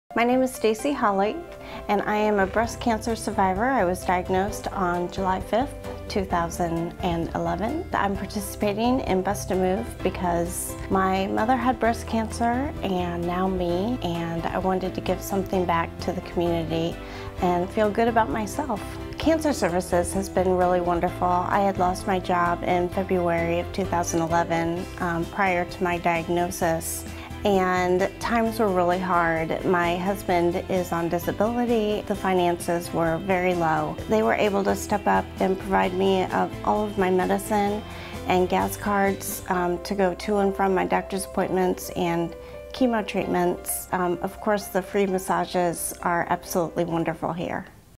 Breast Cancer Survivor Stacie Holley Shares Her Story
Stacie Holley is one of the castees for this year's Bust A Move, a fundraiser for women with breast cancer in our community